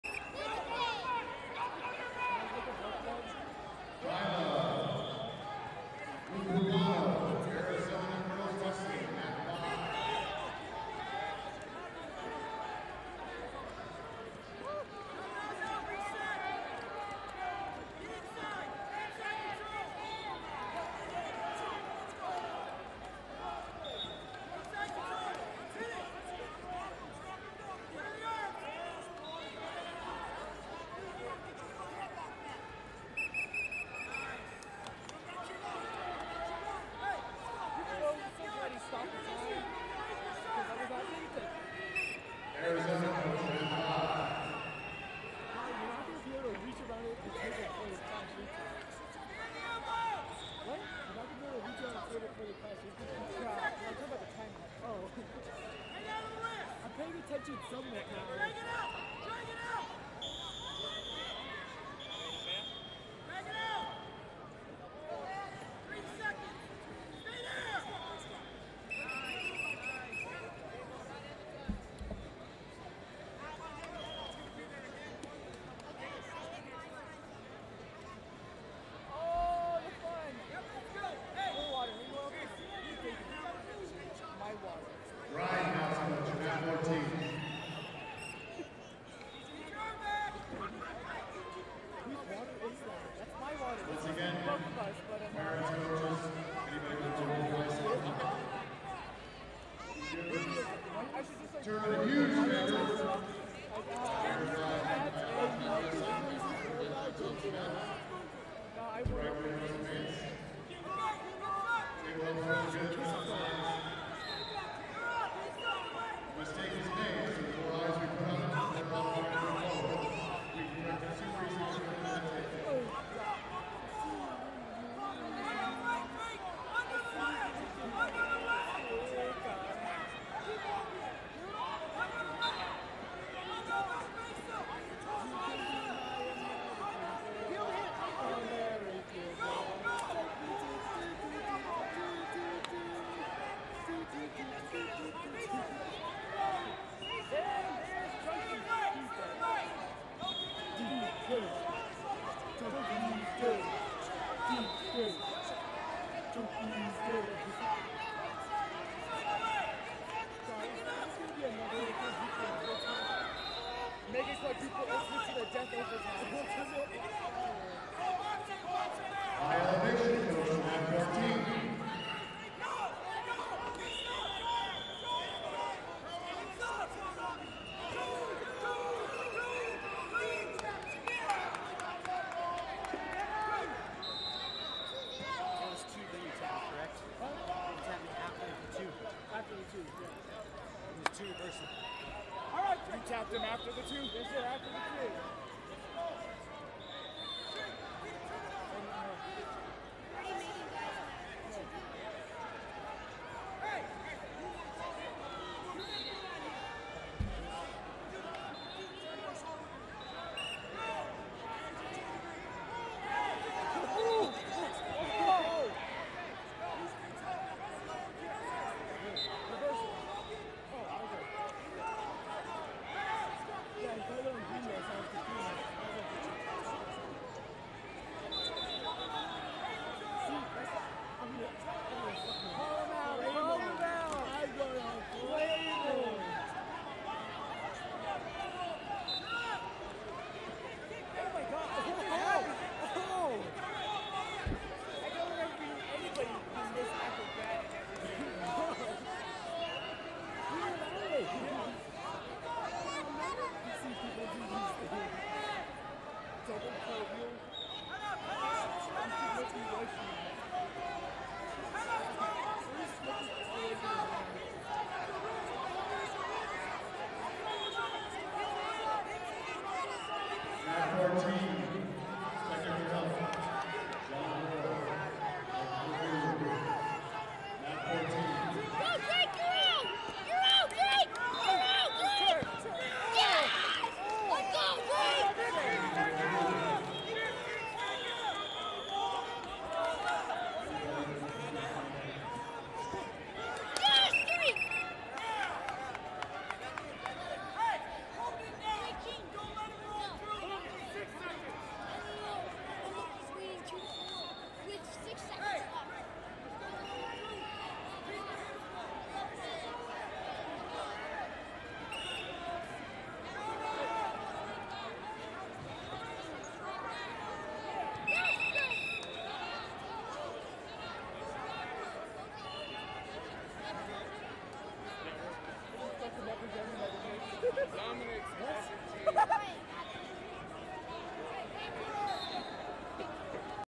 12U 63 Logan Dellow Pomona Elite Vs Drake Jayne Legacy Wrestling Academy
2019 King Of The Mountain - 12U 63 Logan Dellow Pomona Elite Vs Drake Jayne Legacy Wrestling Academy